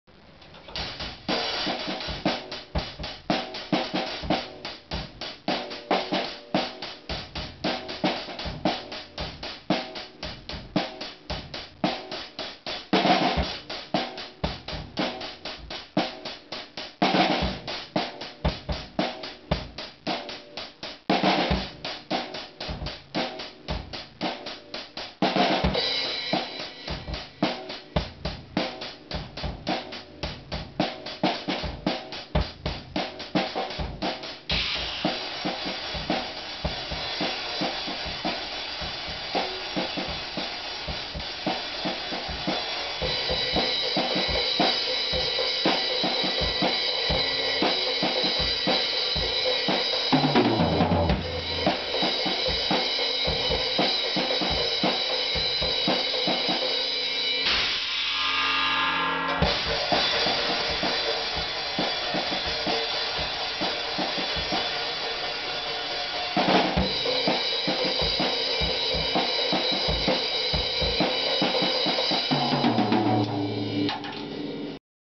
Drumming Fanatic
This is a video of me drumming. Uhhh... Yeah. This is on a Pearl Kit with Paiste and Zildjian cymbals and a Sabian hihat